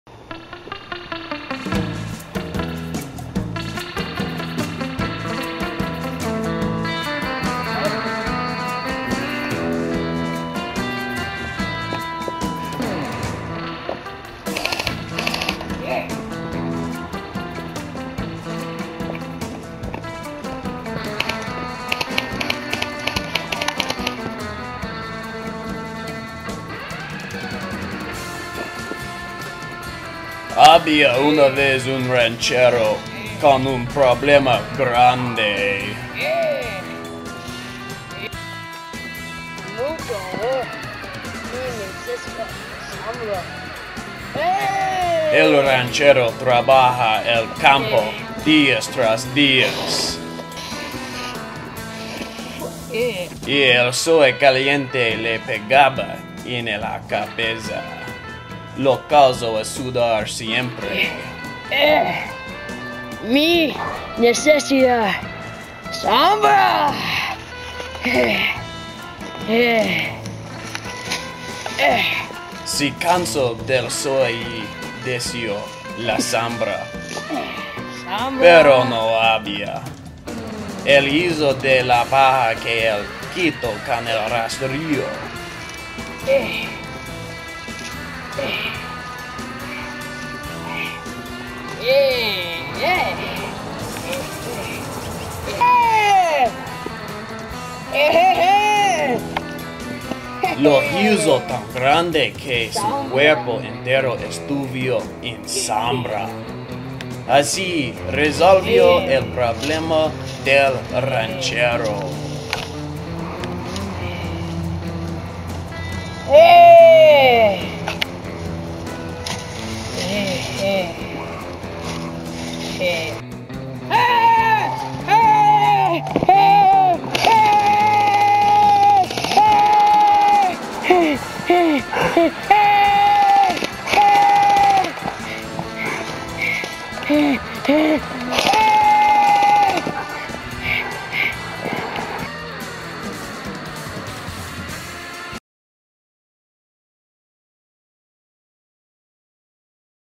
The history of the sombrero
the wonderful horrible life of the ranchero.
how the sombrero came to be..